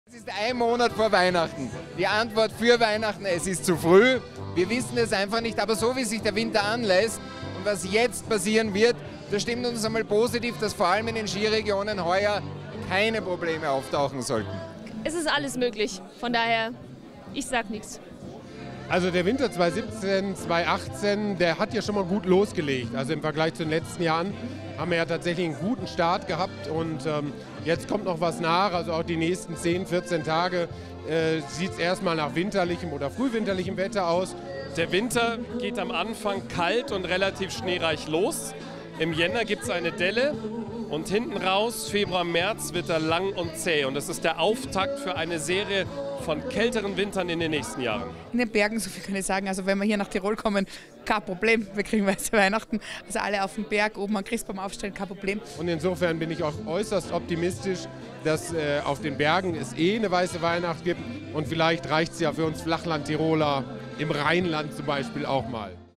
11. Internationaler Wettergipfel in Obergurgl-Hochgurgl: So wird der Winter 2017/2018
Vom 23. bis 25. November 2017 trafen sich zum bereits 11. Mal WettermoderatorInnen aus ganz Europa zum Internationalen Wettergipfel im hinteren Ötztal. Ihre Wetterprognose für den anstehenden Winter 2017/2018 gibt's in diesem Video.

From 23rd to 25th November 2017, television weather presenters from all over Europe gathered in Obergurgl-Hochgurgl for the 11th International Weather Summit. In this video, they present their forecast for the upcoming winter season 2017/2018

Featured in this video in order of appearance:

Locations:

Hohe Mut Alm Obergurgl-Hochgurgl

People:

Marcus Wadsak, Susanne Schöne, Björn Alexander, Kai Zorn, Verena Schneider,

Credits
Idea & Concept: pro media kommunikation gmbh
Director & Produzent: pro media kommunikation gmbh
Camera: pro media kommunikation gmbh
Cut: pro media kommunikation gmbh